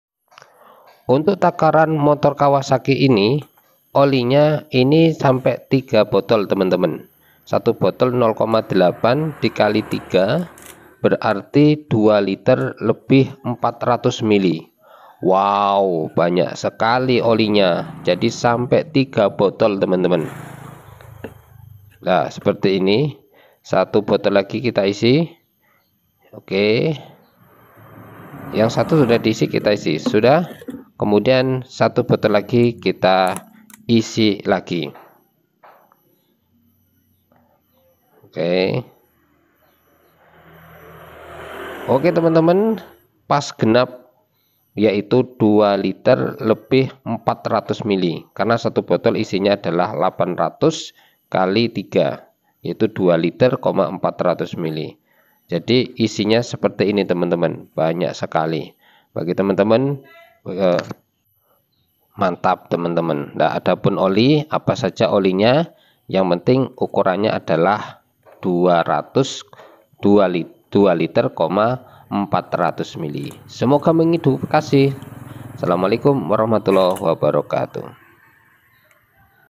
3 botol...takaran oli untuk motor Kawasaki
assalamualaikum warahmatullah wabarakatuh pada video kali ini saya memberikan tutorial yaitu untuk jenis tipe motor Kawasaki bahwa oli takarannya sekitar 3 botol atau 2 liter lebih 400 mili